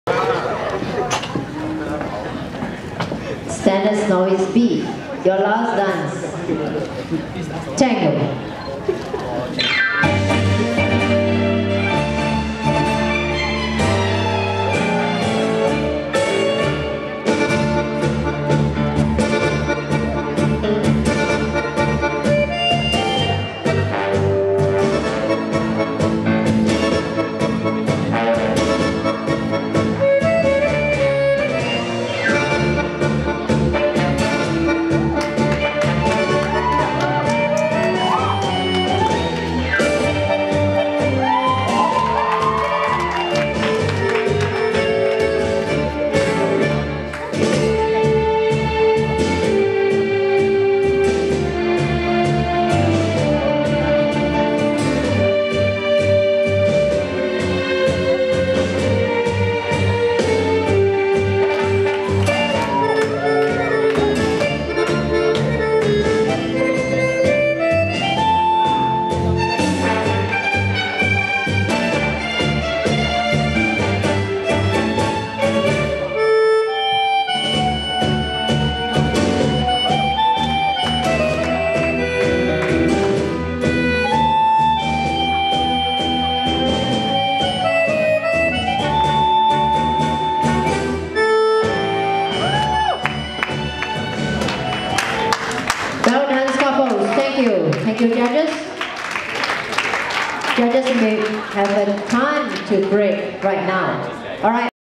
Stars of Tomorrow 2016 Standard Novice B Tango Finals
7TH STARS OF TOMORROW INTERNATIONAL BALLROOM DANCING CHAMPIONSHIP

24th January 2016 at Serangoon Gardens Country Club